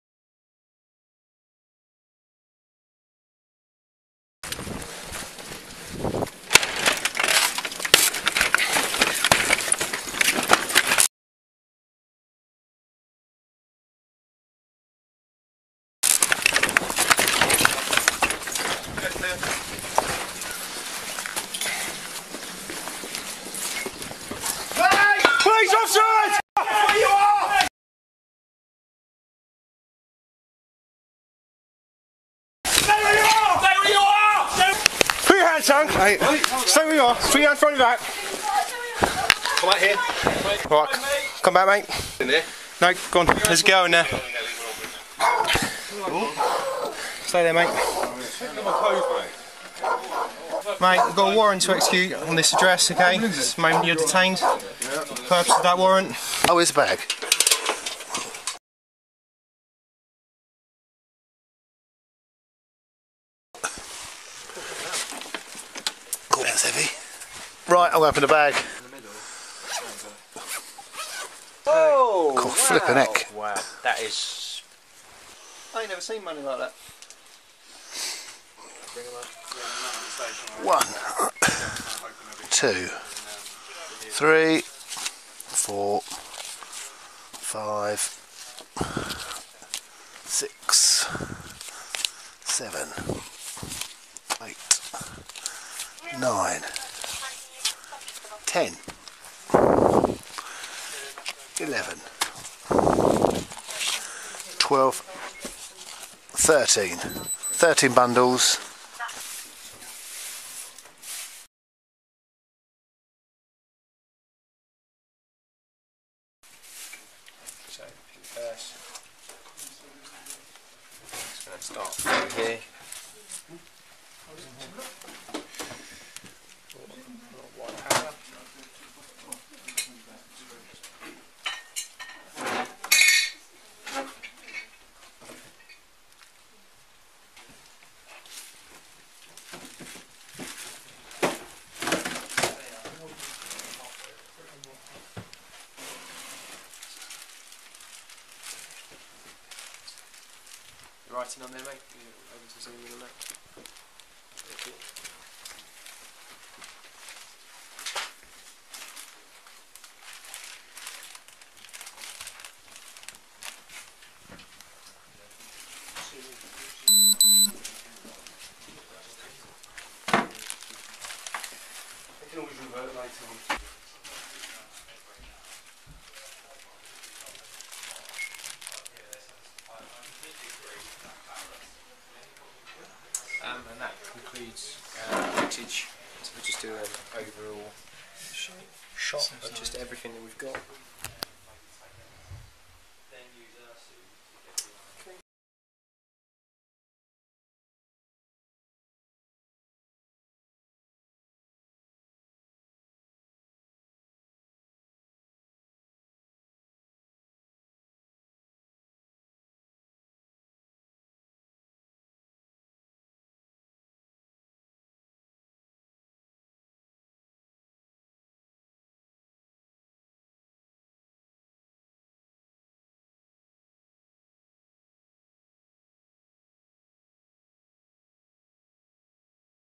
Kent Police Raid Uncovers Large Amount Of Drugs and Over £50,000
--- Case Details --

A drug dealer has been jailed after officers found more than £50,000 hidden inside a chimney.

Samuel Overy, 19, from Chatham, was jailed for four years and four months at Maidstone Crown Court on 27 July 2020.

Overy had pleaded guilty to possession with intent to supply cocaine, possession with intent to supply cannabis and acquiring criminal property.

He was arrested on 12 March 2020 following a search warrant at a house in Solomon Road, Gillingham, where a safe was found containing cocaine and cannabis. A mobile phone was also found which contained messages related to the selling of drugs.

Overy was at the address during the search and admitted the drugs and phone belonged to him. A further search warrant was carried out at his home address in Magpie Hall Road.

During the search officers found a black bag hidden inside a chimney and inside the bag police found £54,650 which Overy admitted was the cash he had made from selling drugs.

He was arrested and subsequently charged with the offences.

As well as being sentenced the judge also issued a forfeiture and destruction notice for all drugs, cash and paraphernalia.

-- End Of Case Details --

-- Tags (Ignore) --
kent police
chatham
gillingham
drugs
weed
cocaine
cannabis
raid
arrest
hidden
bust
money
cash
stash
phone
stolen